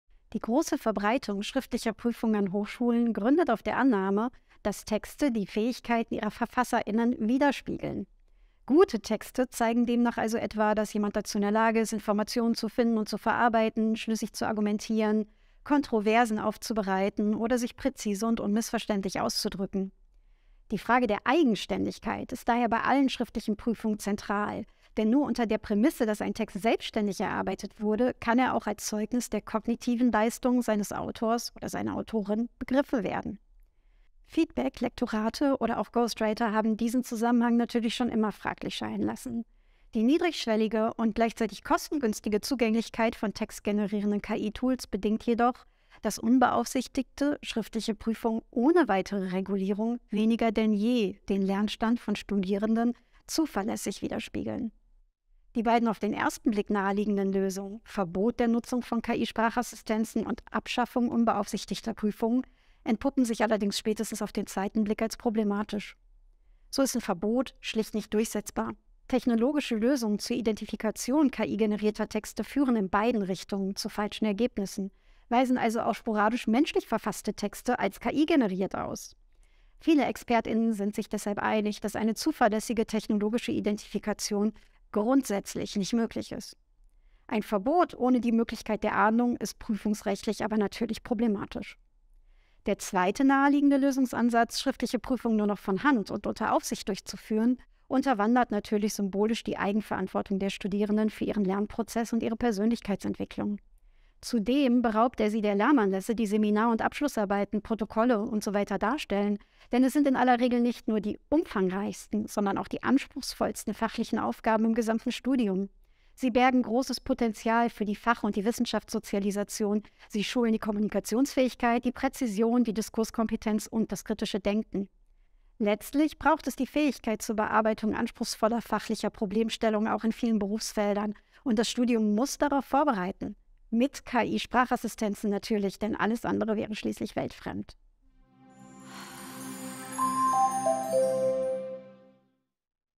Wie beeinflussen KI-Sprachassistenzen Prüfungen?
Anika Limburg (Hochschule RheinMain) beschreibt den Einfluss von textgenerierenden KI-Tools auf schriftliche Prüfungsleistungen sowie vermeintliche Lösungen für die Hochschullehre.

Das Video stammt aus dem dritten Modul des Online-Kurses "Sprachassistenzen als Chance für die Hochschullehre". In dem Kurs geht es um Implikationen für die Prüfungskultur, die sich aus dem Einsatz von Sprachassistenzen in der Lehre ergeben. Ihr lernt, KI-Sprachmodelle produktiv, aber auch kritisch reflektiert an der Hochschule zu nutzen.

Der KI-Campus ist ein vom Bundesministerium für Bildung und Forschung (BMBF) gefördertes F&E-Projekt. Er ist seit Juli 2020 als Beta-Version öffentlich verfügbar.